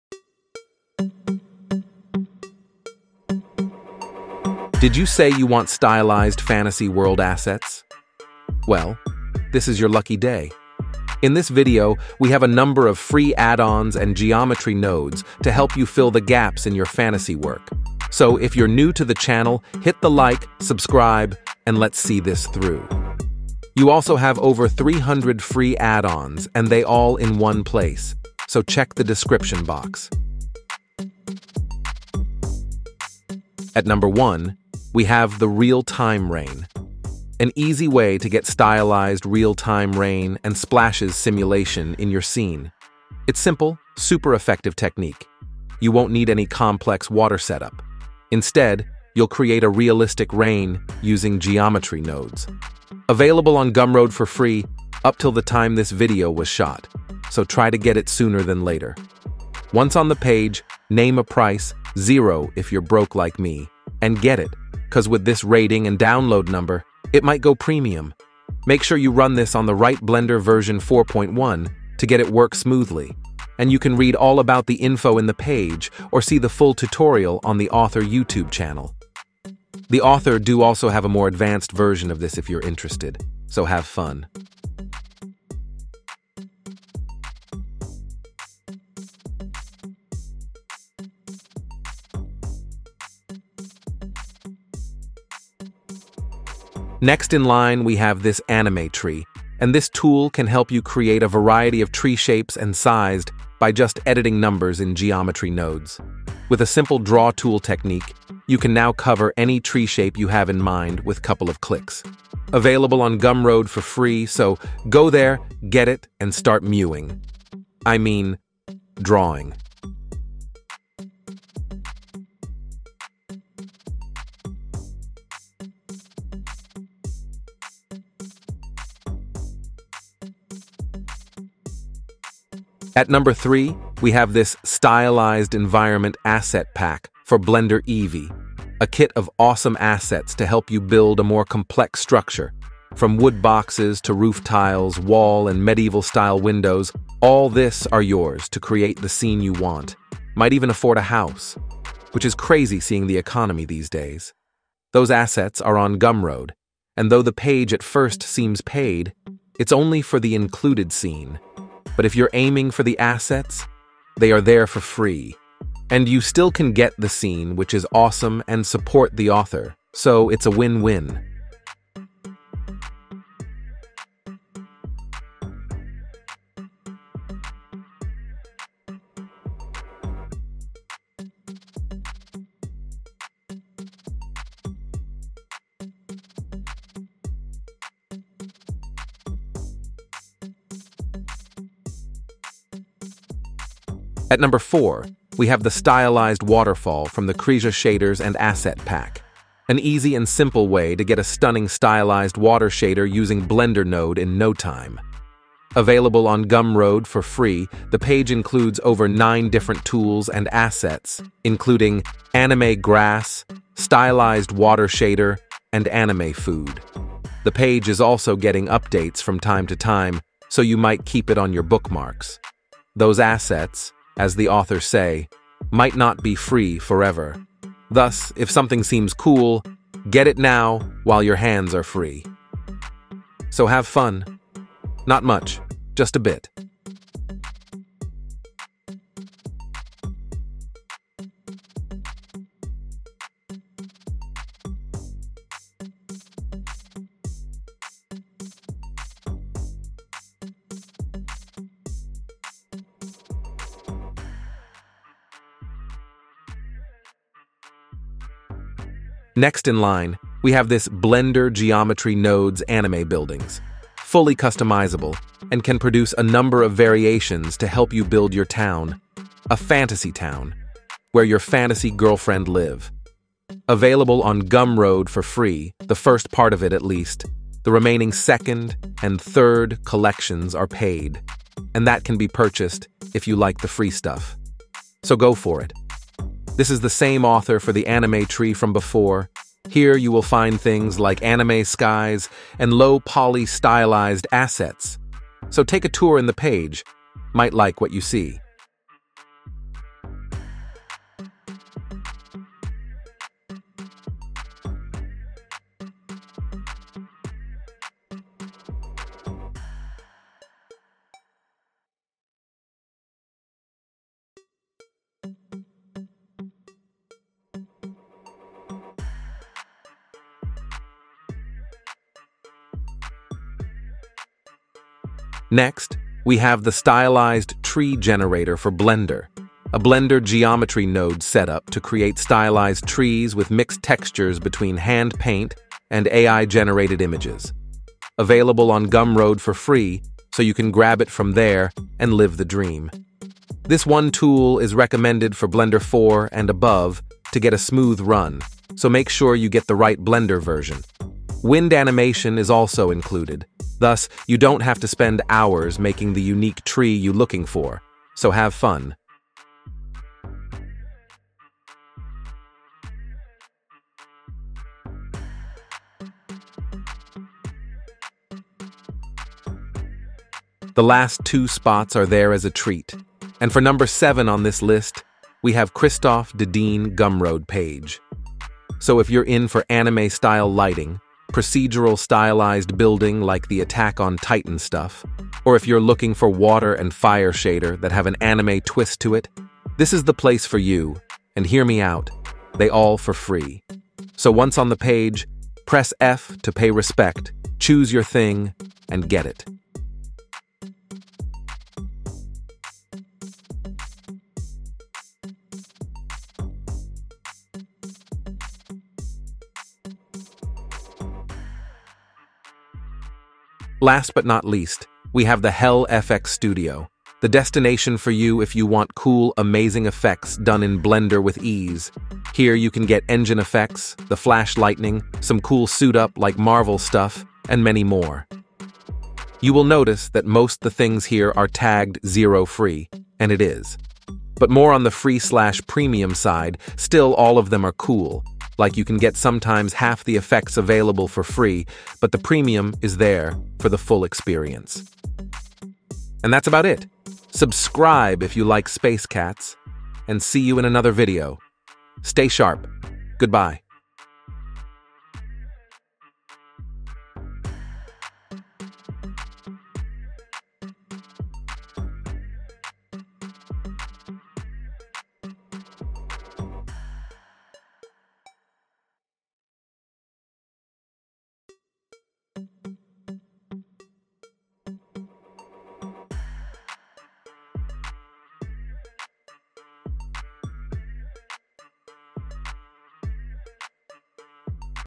Free Blender Addons INSANELY Good For ALL!
0:00 Introduction
0:33  Rain and Splashes
01:36 GHIBLI&ANIME TREE
02:11 Stylized Environment asset pack
03:12 Stylized Water
04:18 Anime Building
05:24 Stylized tree generator
06:10 Kristof Dedene
06:53 Hell FX